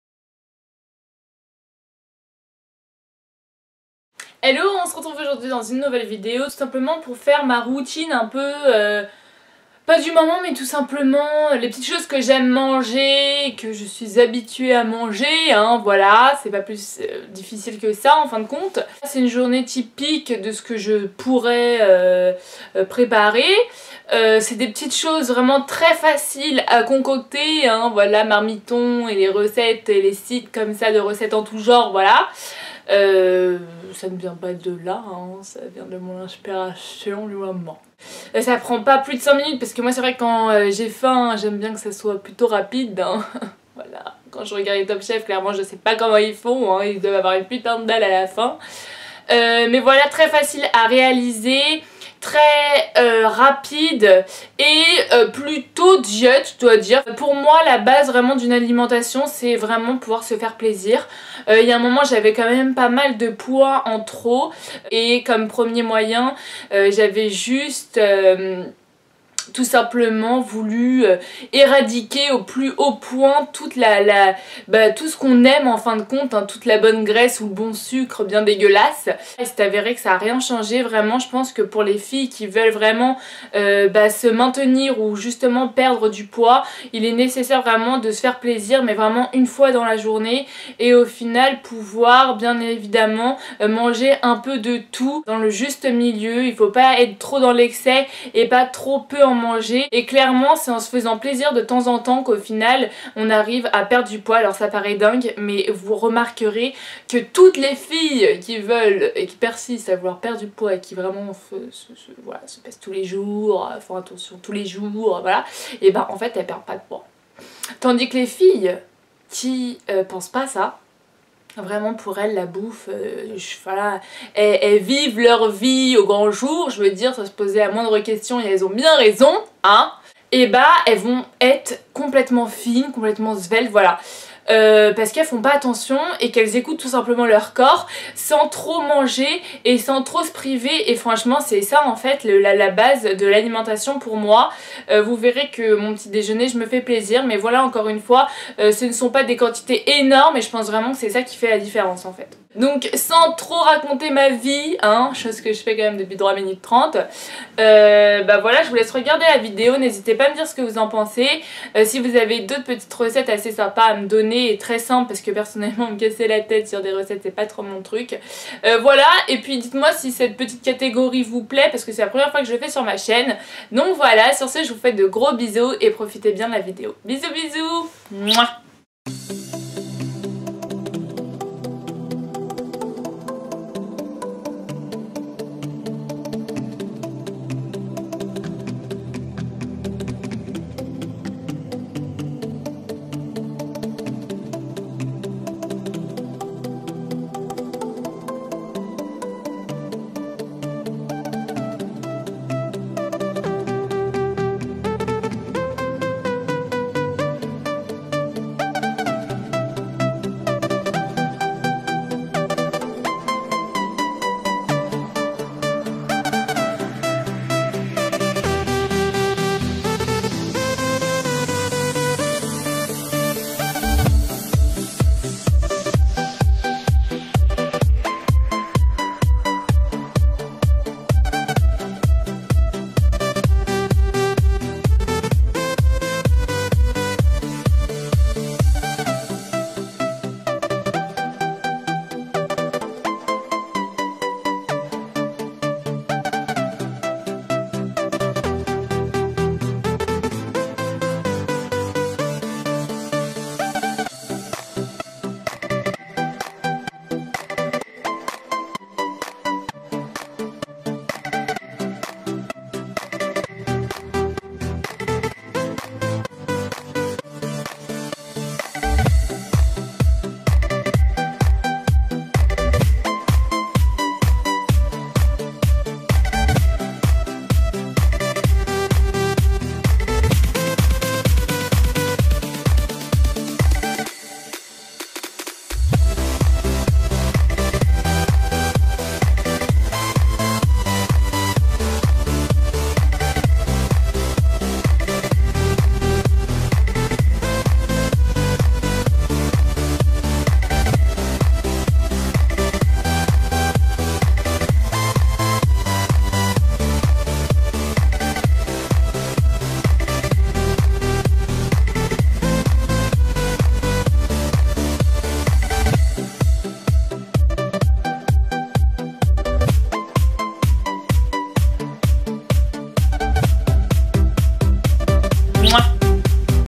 Une journée dans mon assiette !!! HEALTHY & RAPIDE ♥
DEROULE LA BARRE POUR PLUS D'INFOS:

Hellllo los amigos de las noche, une petite vidéo pour vous montrer un peu mes petites habitudes niveau alimentation voili voilou dîtes moi si ça vous plait enfin ce genre de vidéo hein j'entends bien !!! Sur ceux je vous fais de gros bisous et à très vite. Mouahhhhh